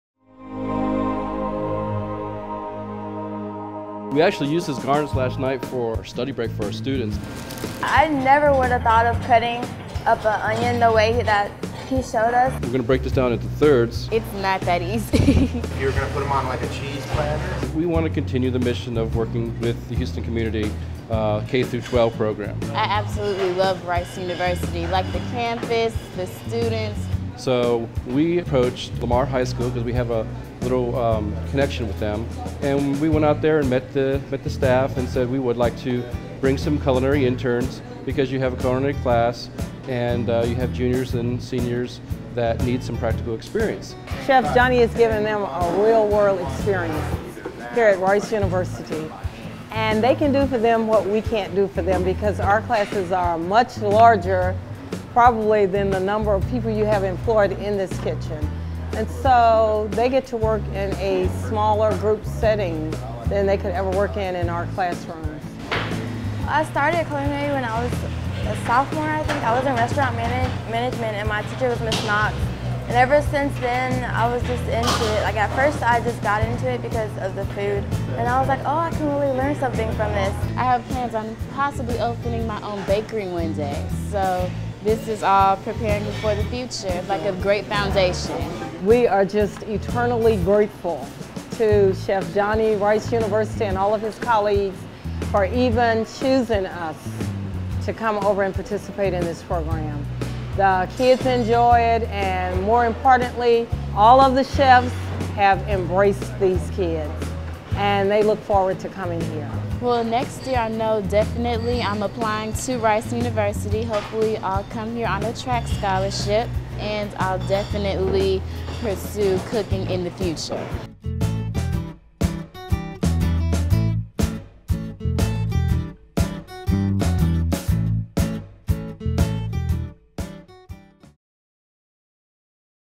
Houston high school students turn food into art at Rice University
A unique partnership between Rice University Dining Services and Lamar High School is giving students an up-close look at what it takes to run a busy commercial kitchen and teaching them the practices of the culinary industry. Lamar students come to the Rice campus several times a week and work closely with the executive chefs in the serveries. The students get academic credit through the program, get paid for the work they do on campus and when the interns graduate, they will receive a city of Houston Food Service Manager certificate. Not only does the program give students who are considering culinary careers a better understanding of the field, but it also creates a potential entry-level pool of talent for the university. Even more important, this program supports the Vision for the Second Century "to fully engage with the city of Houston -- learning from it and contributing to it -- as a successful partnership with our home city as an essential part of our future."